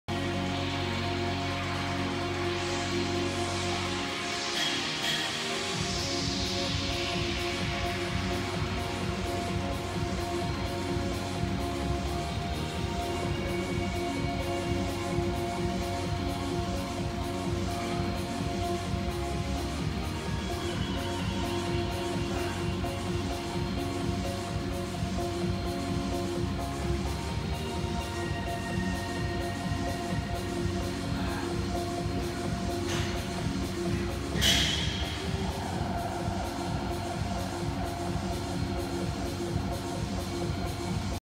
1 Arm DB Tricep Extension
Join My Revolt Fitness App Free Community Revolt Fitness - Tricep Exercise to bias the long head of the Tricep